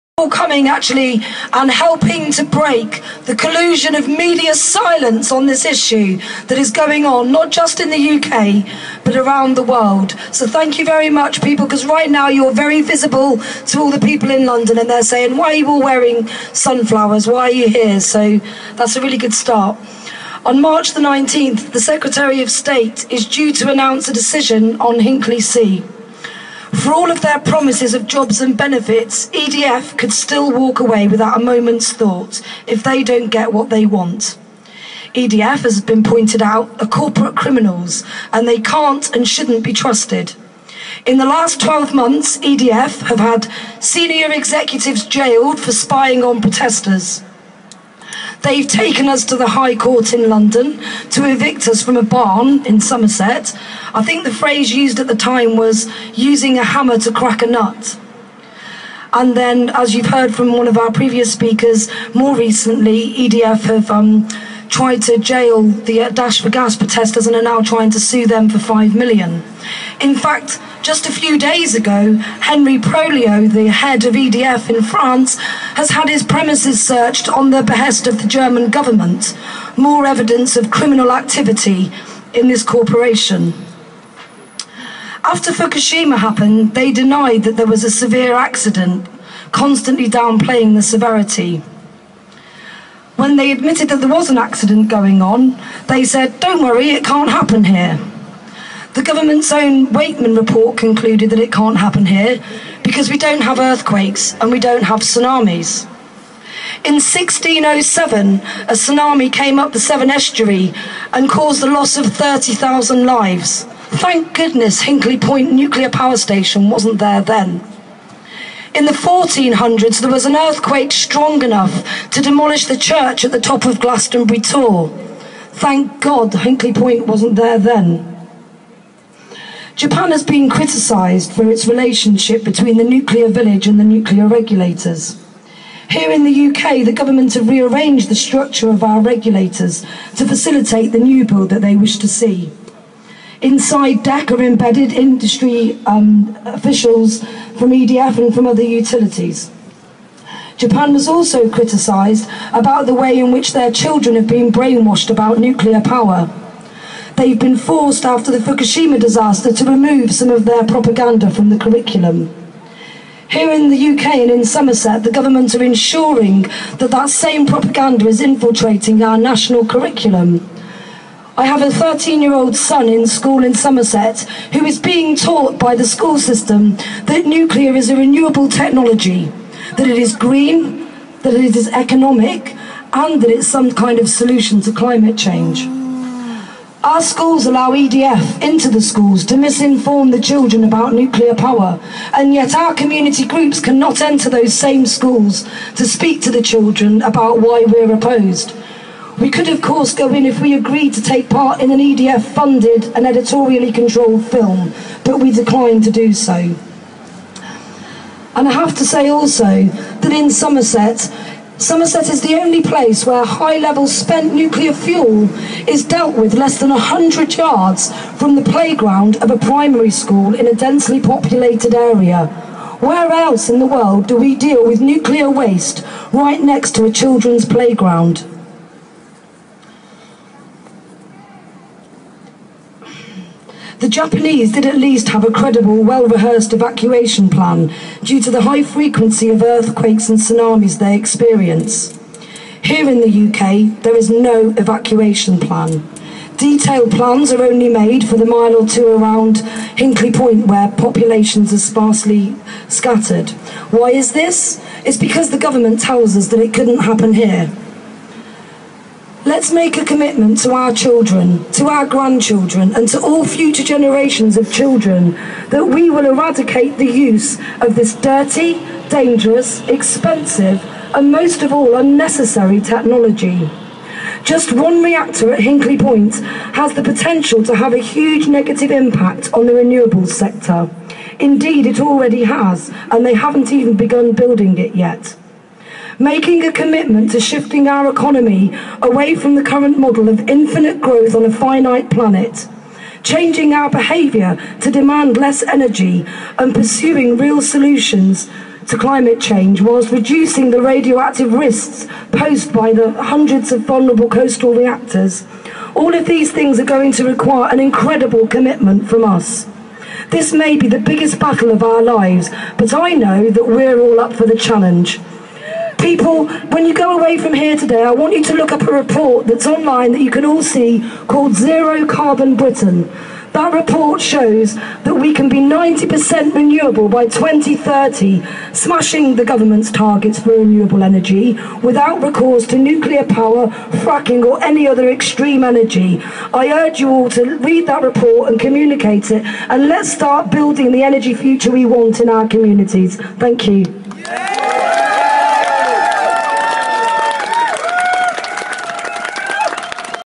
Remember Fukushima! NO to Nuclear Power! 2013 UK Parliament (JAN UK)
Nikki Clark, South West Against Nuclear